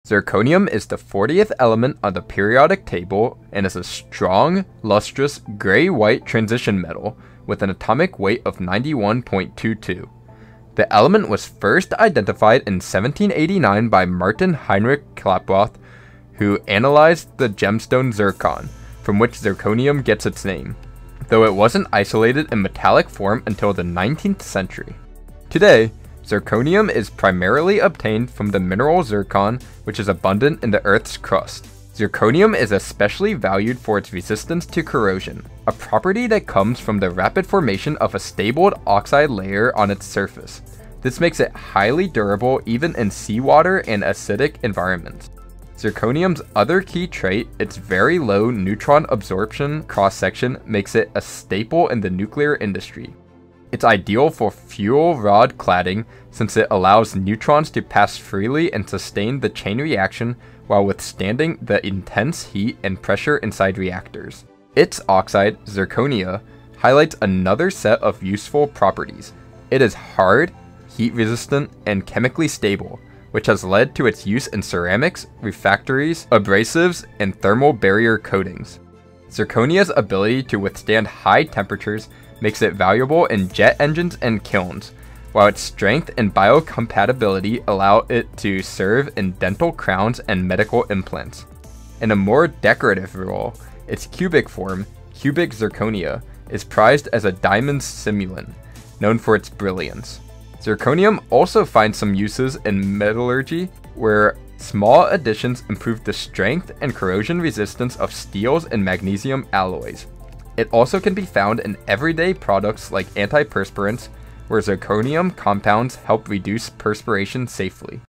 Zirconium in 100 Seconds
Hey hey! Quick video summary about fortieth element on periodic table zirconium. New episode in series where I am giving a quick summary of every element on the periodic table.